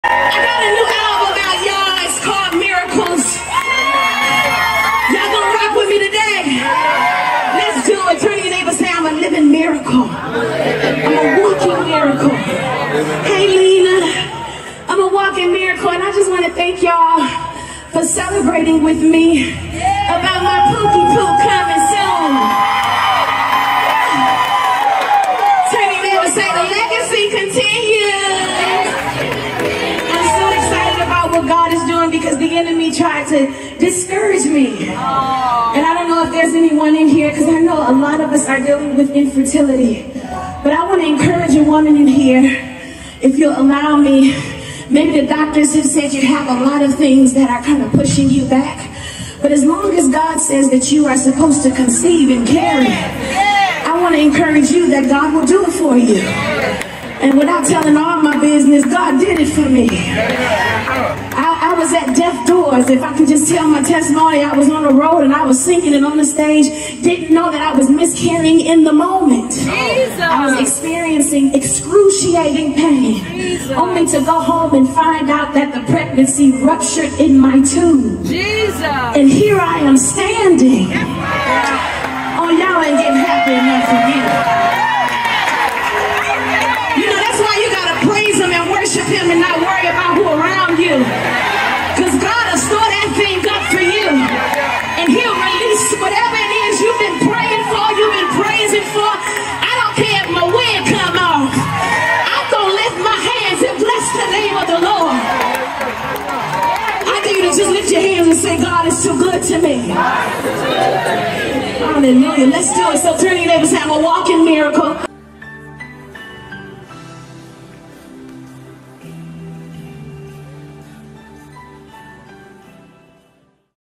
Kierra Sheard Kelly From Miscarriage To Miracle Baby - The Legacy Continues!
Kierra gives powerful testimony of how God brought her through a miscarriage and into her present miracle pregnancy now.  THE LEGACY CONTINUES!

Bible Verse: Behold I will do a new thing now it shall spring forth shall ye not know it? I will even make a way in the wilderness and rivers in the desert.